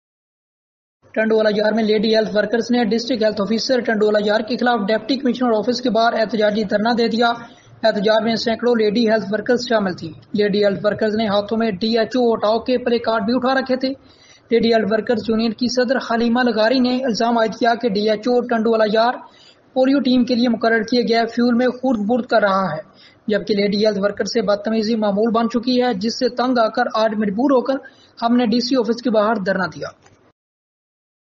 Hundreds Of Lady Health Workers In Tandoalahyar Were Mistreated By DHO | Voice Hayderabad
Hundreds Of Lady Health Workers In Tandoalahyar Were Mistreated By District Health Officer | Voice Hayderabad